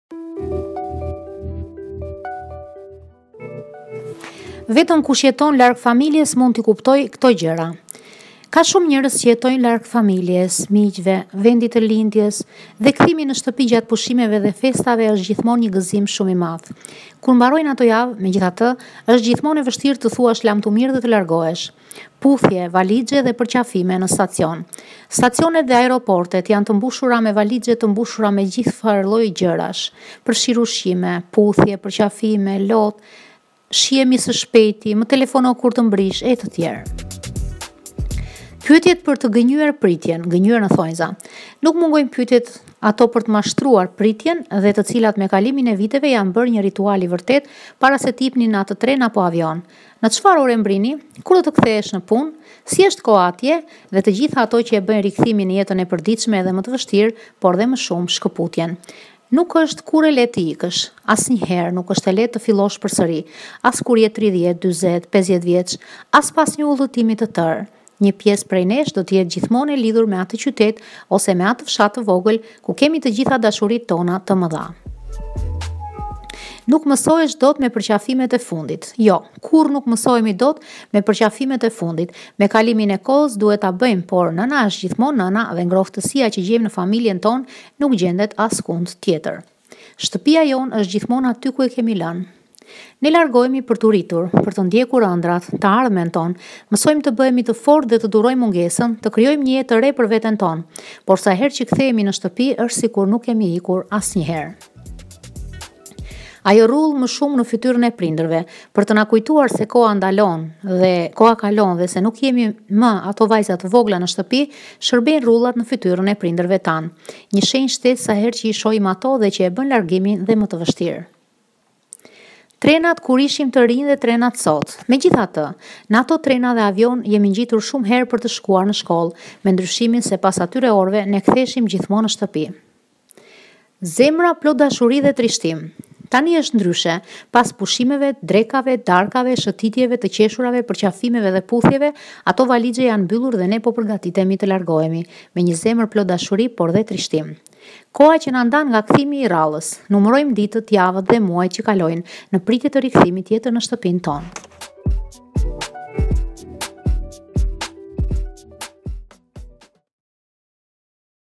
Vetëm kush jeton larg familjes, mund t’i kuptojë këto gjëra
Ka shumë njerëz që jetojnë larg familjes, miqve, vendit të lindjes dhe kthimi në shtëpi gjatë pushimeve dhe festave është gjithmonë një gëzim i madh. Kur mbarojnë ato javë, megjithatë, është gjithmonë e vështirë të thuash lamtumirë dhe të largohesh.

Puthje, valixhe dhe përqafime në stacion
Stacionet dhe aeroportet janë të mbushura me valixhe të mbushura me gjithfarësoj gjërash, përfshirë ushqime, por edhe puthje, përqafime, lot, të “shihemi së shpejti”, dhe “më telefono kur të mbërrish”.

Pyetjet për të “gënjyer” pritjen
Nuk mungojnë pyetjet, ato për të mashtruar pritjen dhe të cilat, me kalimin e viteve, janë bërë një ritual i vërtetë, para se të hipni në atë tren apo avion. “Në çfarë ore mbërrin?”, “Kur do të kthehesh në punë?”, “Si është koha atje?”. Dhe të gjitha ato që e bëjnë rikthimin në jetën e përditshme edhe më të vështirë, por edhe më shumë shkëputjen.

Nuk është kurrë e lehtë të ikësh
Asnjëherë nuk është e lehtë të fillosh përsëri. As kur je 30, 40, 50 vjeç, as pas një udhëtimi të tërë. Një pjesë prej nesh do të jetë gjithmonë e lidhur me atë qytet ose me atë fshat të vogël, ku kemi të gjitha dashuritë tona më të mëdha.

Nuk mësohesh dot me përqafimet e fundit
Jo, kurrë nuk mësohemi dot me përqafimet e fundit. Me kalimin e kohës duhet ta bëjmë, por nëna është gjithmonë nëna dhe ngrohtësia që gjejmë në familjen tonë, nuk gjendet askund tjetër.

Shtëpia jonë është gjithmonë aty ku e kemi lënë
Ne largohemi për t’u rritur, për të ndjekur ëndrrat, të ardhmen tonë. Mësojmë të bëhemi të fortë dhe të durojmë mungesën, të krijojmë një jetë të re për veten tonë. Por sa herë që kthehemi në shtëpi, është sikur nuk kemi ikur asnjëherë.

Ajo rudhë më shumë në fytyrën e prindërve …
Për të na kujtuar se koha kalon dhe se nuk jemi më ato vajza të vogla në shtëpi, shërbejnë rrudhat në fytyrat e prindërve tanë. Një shenjë shtesë sa herë që i shohim ato dhe që e bën largimin edhe më të vështirë.

Trenat kur ishim të rinj, dhe trenat sot
Megjithatë, në ato trena e avionë jemi ngjitur shumë herë për të shkuar në shkollë, me ndryshimin se pas atyre orëve ne ktheheshim gjithmonë në shtëpi.

Zemra plot dashuri dhe trishtim
Tani është ndryshe, pas pushimeve, drekave, darkave, shëtitjeve, të qeshurave, përqafimeve dhe puthjeve, ato valixhe janë mbyllur dhe ne po përgatitemi të largohemi. Me një zemër plot dashuri, por edhe trishtim.

Koha që na ndan nga kthimi i radhës
Numërojmë ditët, javët dhe muajt që kalojnë, në pritje të rikthimit tjetër në shtëpinë tonë